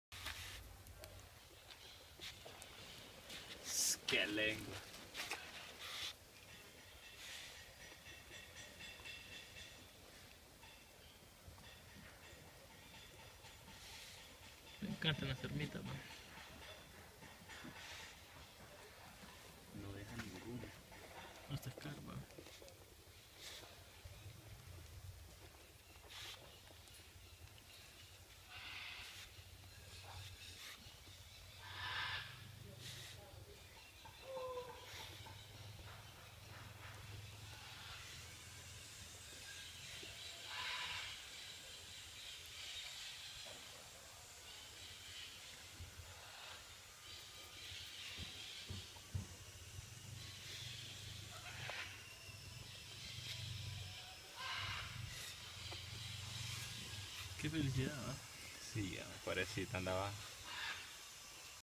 oso hormiguero (Tamandua mexicana), Zoológico Rosy Walther Honduras
Un oso hormiguero comiendo termitas